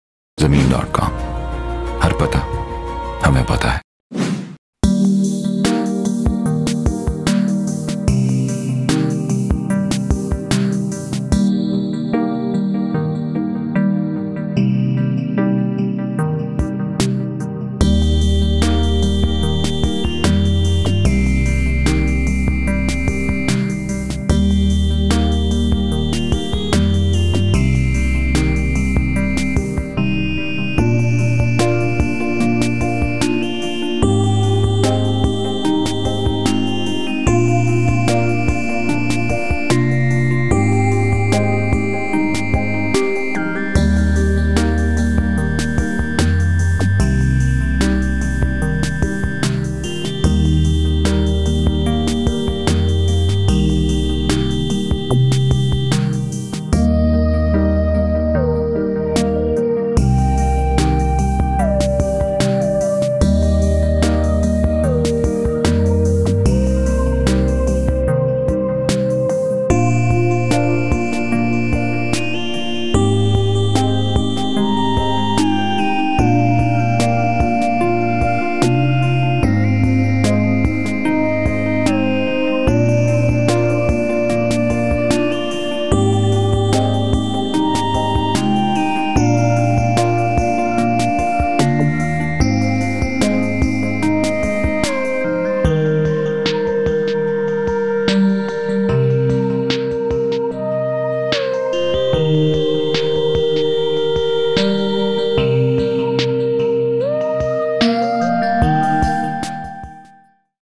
6 MARLA LOWER PORTION FOR RENT IN PHASE 8 DHA KARACHI
New Portion, 150 Yards With Big Front, Ground Floor, 2 Bed Lounge, Tile Flooring, Washing Area, Powder, Full Basement, Parking. Peace Full Vicinity. Only In Rs 75000, Near Creek Vista Phase 8.